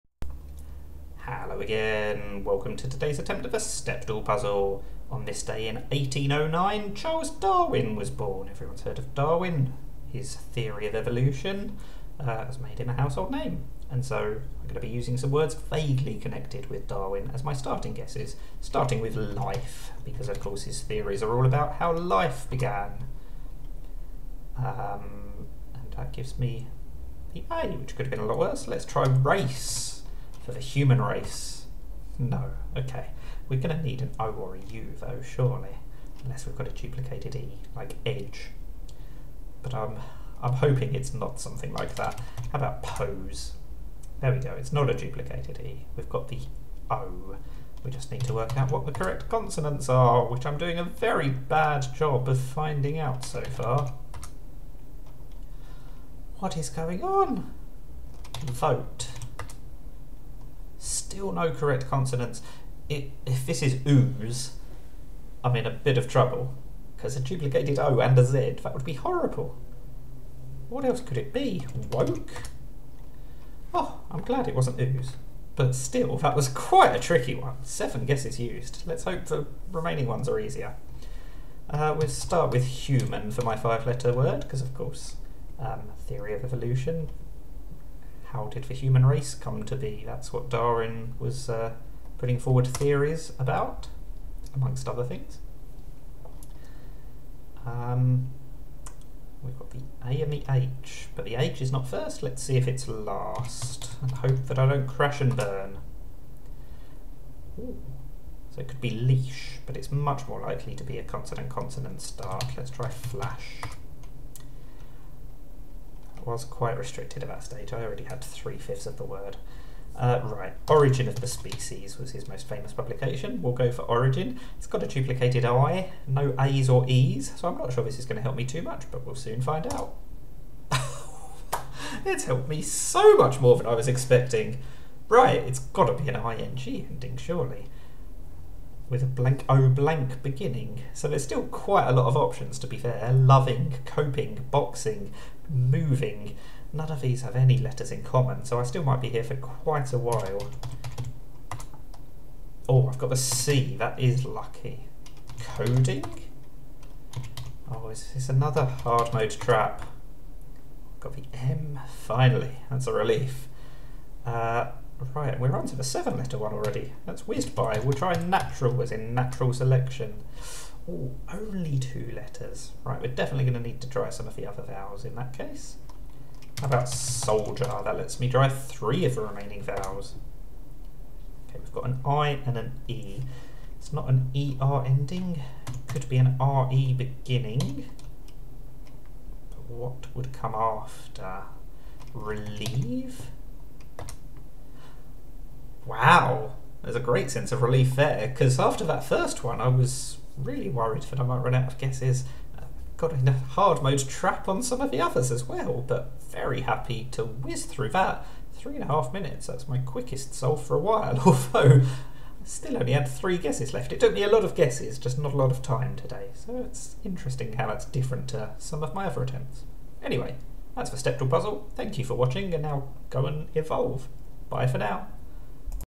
A STEPDLE puzzle for the birthday of CHARLES DARWIN!
Stepdle Wordle puzzle for 12 Feb 24

Today I attempt a wordle variant called Stepdle. There are 4 Wordle puzzles of different lengths, each a longer word than the one before!